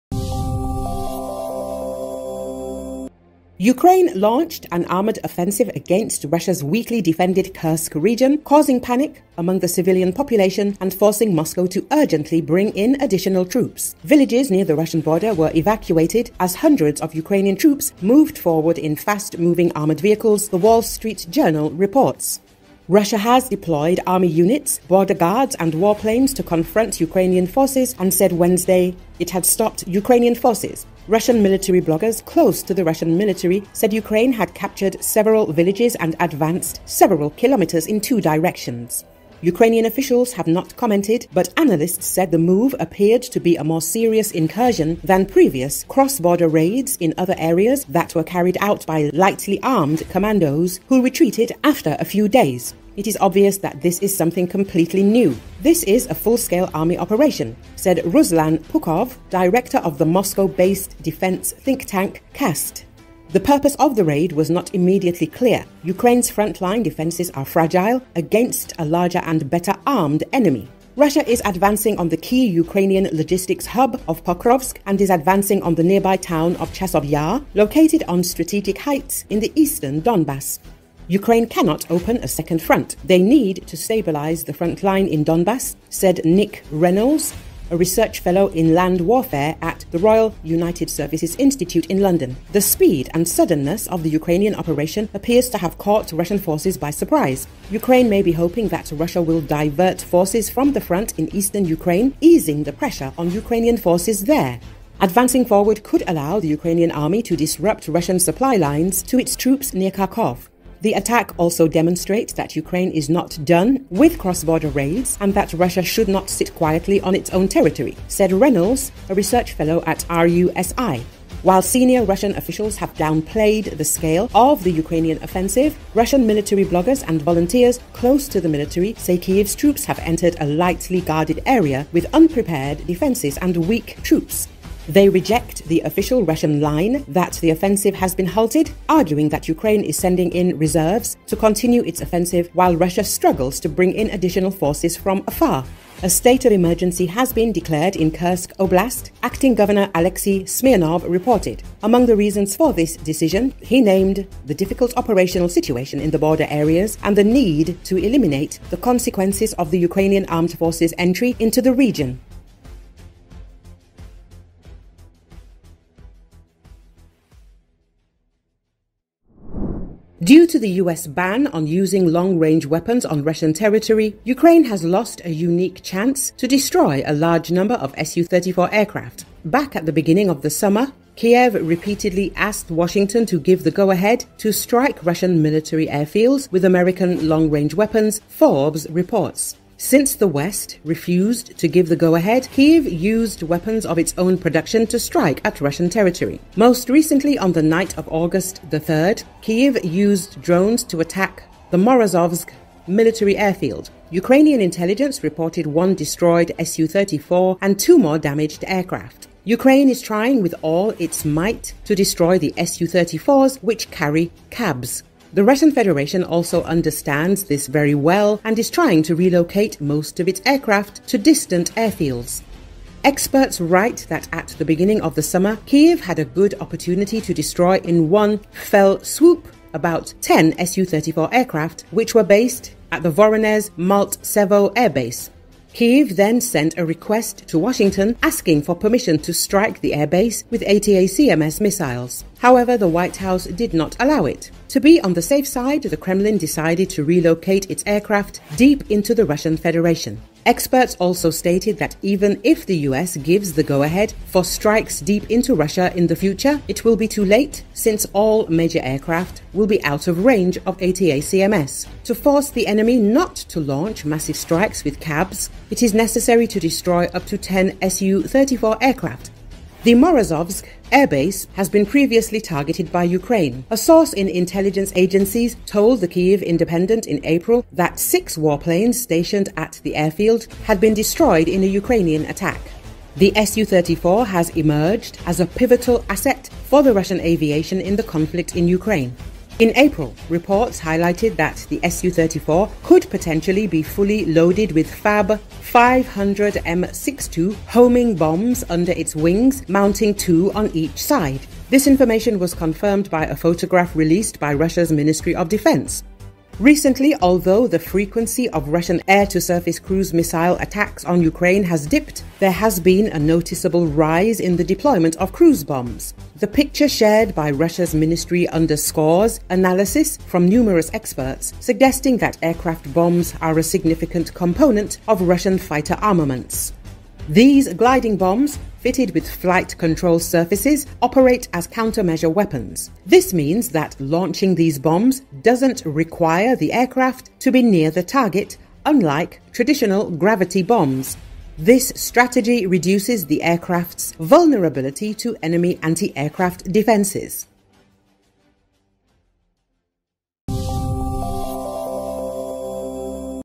Panic among residents in Kursk, villages are being emptied, thousands of residents are evacuated
Ukraine launched an armored offensive against Russia's weakly defended Kursk region, causing panic among the civilian population and forcing Moscow to urgently bring in additional troops. Villages near the Russian border were evacuated as hundreds of Ukrainian troops moved forward in fast-moving armored vehicles, The Wall Street Journal reports .
Russia has deployed army units, border guards and warplanes to confront Ukrainian forces and said Wednesday it had stopped Ukrainian forces. Russian military bloggers close to the Russian military said Ukraine had captured several villages and advanced several kilometers in two directions.

Ukrainian officials have not commented, but analysts said the move appeared to be a more serious incursion than previous cross-border raids in other areas that were carried out by lightly armed commandos who retreated after a few days.

"It is obvious that this is something completely new. This is a full-scale army operation," said Ruslan Pukhov, director of the Moscow-based defense think tank CAST.

The purpose of the raid was not immediately clear. Ukraine's front-line defenses are fragile against a larger and better-armed enemy.

Russia is advancing on the key Ukrainian logistics hub of Pokrovsk and is advancing on the nearby town of Chasov Yar, located on strategic heights in the eastern Donbass.

"Ukraine cannot open a second front. They need to stabilise the front line in Donbas," said Nick Reynolds, a research fellow in land warfare at the Royal United Services Institute in London.

The speed and suddenness of the Ukrainian operation appears to have caught Russian forces by surprise. Ukraine may be hoping that Russia will divert forces from the front in eastern Ukraine, easing the pressure on Ukrainian forces there.

Advancing forward could allow the Ukrainian army to disrupt Russian supply lines to its troops near Kharkov.

The attack also demonstrates that "Ukraine is not done with cross-border raids and that Russia should not sit quietly on its own territory," said Reynolds, a research fellow at RUSI.

While senior Russian officials have downplayed the scale of the Ukrainian offensive, Russian military bloggers and volunteers close to the military say Kyiv's troops have entered a lightly guarded area with unprepared defenses and weak troops.

They reject the official Russian line that the offensive has been halted, arguing that Ukraine is sending in reserves to continue its offensive, while Russia struggles to bring in additional forces from afar.

A state of emergency has been declared in Kursk Oblast , Acting Governor Alexey Smirnov reported. Among the reasons for this decision, he named "the difficult operational situation in the border areas" and the need to eliminate the consequences of the Ukrainian Armed Forces' entry into the region.